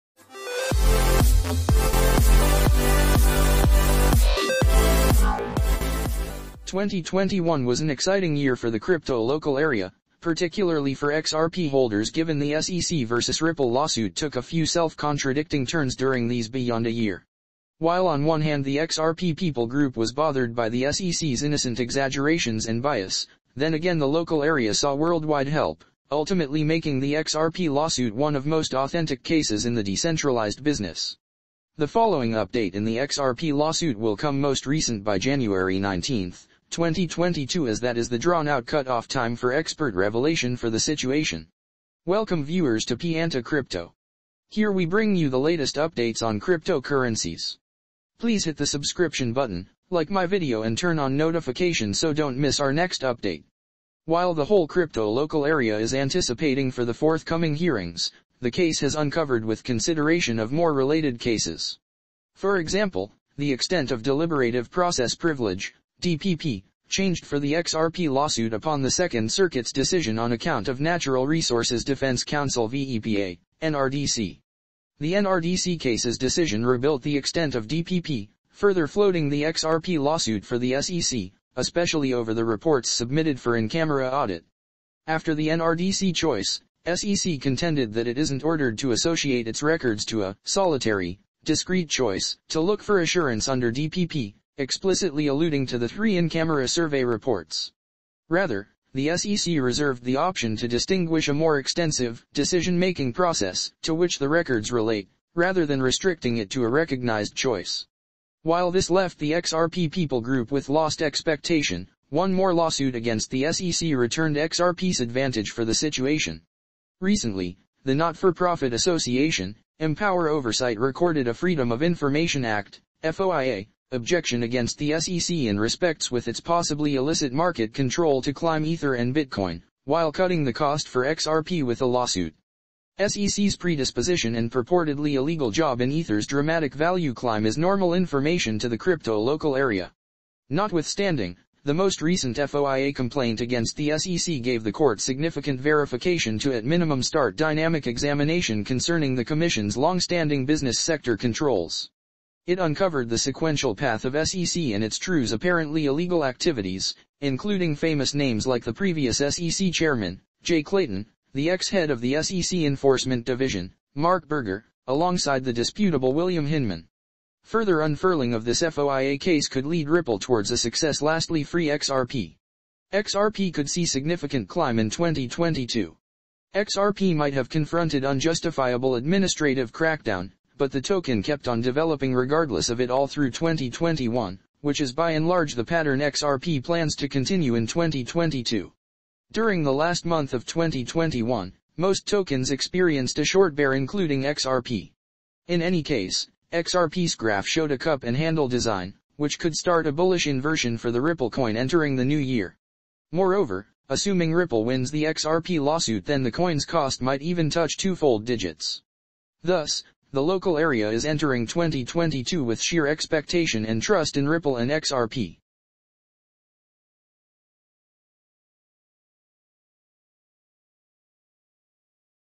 RIPPLE XRP - Brad Garlinghouse and team talk on the ripple lawsuit finale date - ripple news today
RIPPLE XRP

2021 was an eventful year for the crypto community, especially for XRP holders given the SEC vs Ripple lawsuit took several bittersweet turns during these past 12 months. While on one hand the XRP community was aggravated by the SEC’s white lies and prejudice, on the other hand the community saw global support, eventually making the XRP lawsuit one of most historical cases in the decentralised industry. The next update in the XRP lawsuit will come latest by January 19, 2022 as that is the extended deadline for Expert discovery in the case.